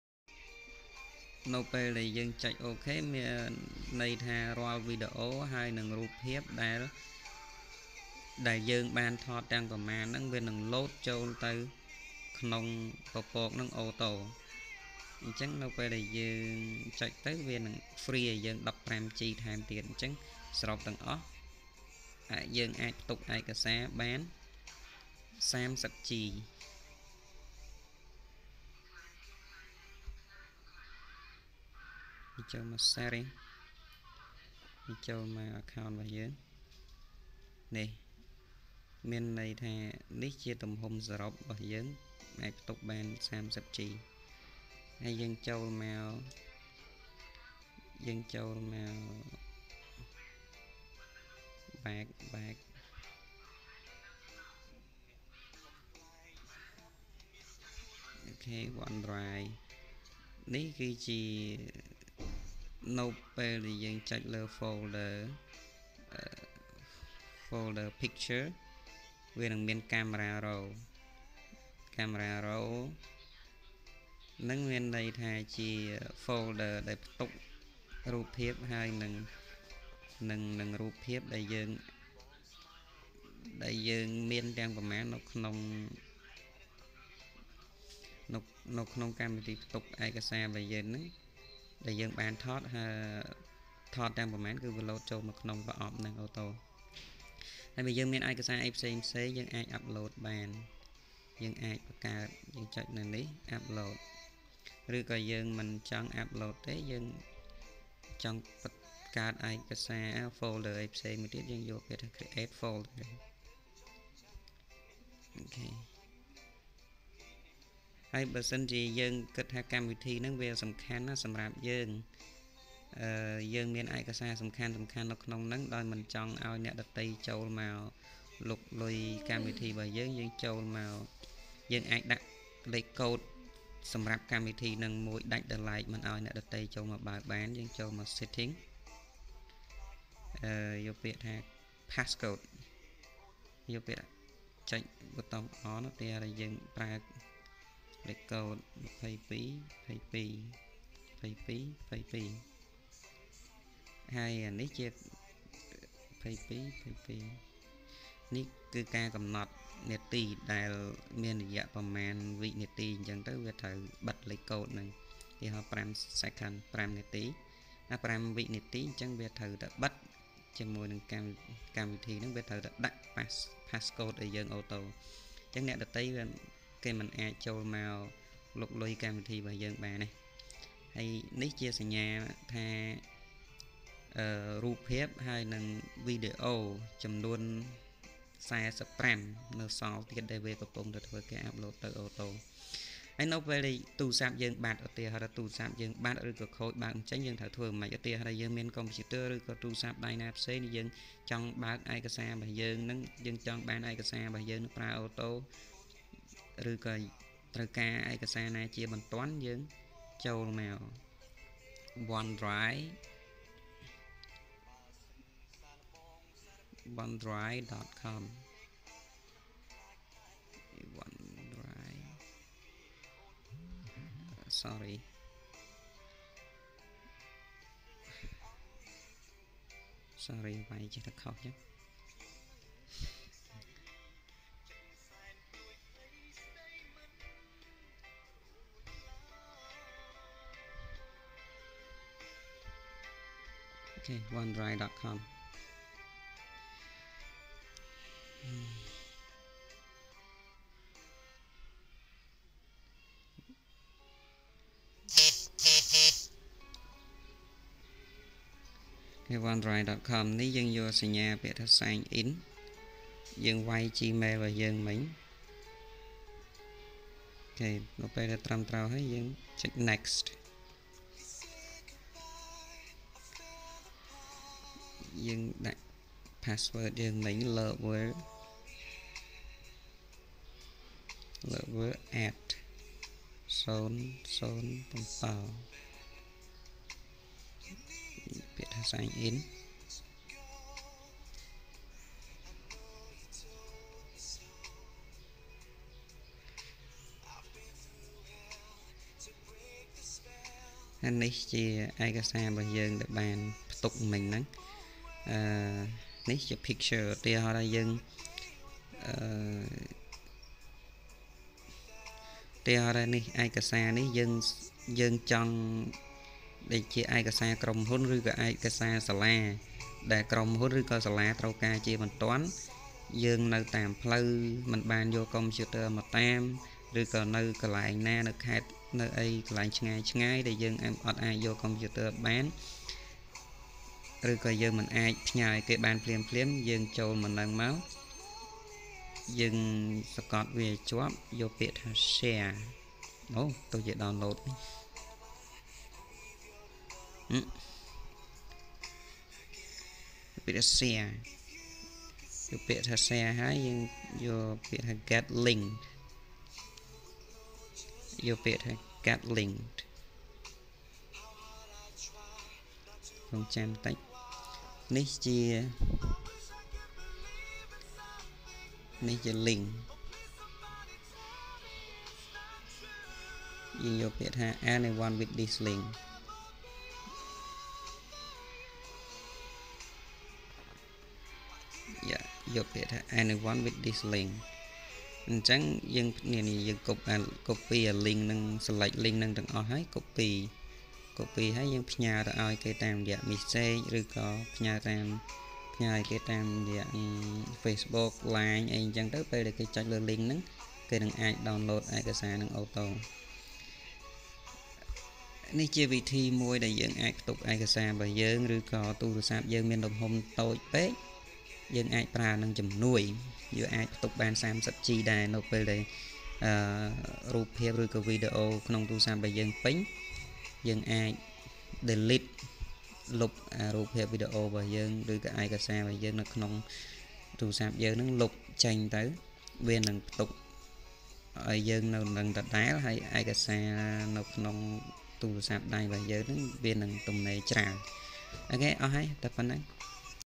How to use OneDrive on your smart phone devices (Part 2)
This video will cover all tips for using OneDrive in your daily life.